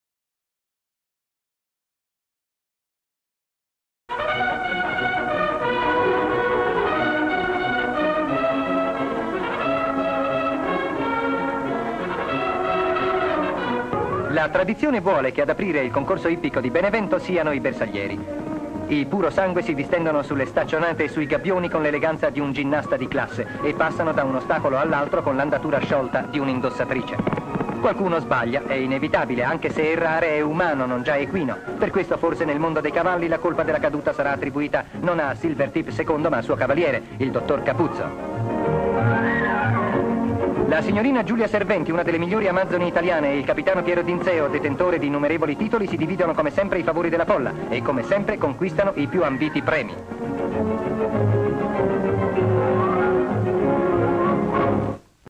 La tradizione
Italia - Concorso Ippico di Benevento. Vittoria di D'Inzeo e di Giulia Serventi
   Descrizione sequenze:Concorso ippico di Benevento, la banda dei bersaglieri percorre un tratto del circuito ippico ; fasi del concorso ippico a ostacoli ; alcuni fantini cadono, la caduta Capuzzo ; la prova di Giulia Serventi ; la prova di D'Inzeo ; il pubblico durante le gare ; i vincitori si recano a ricevere i premi ;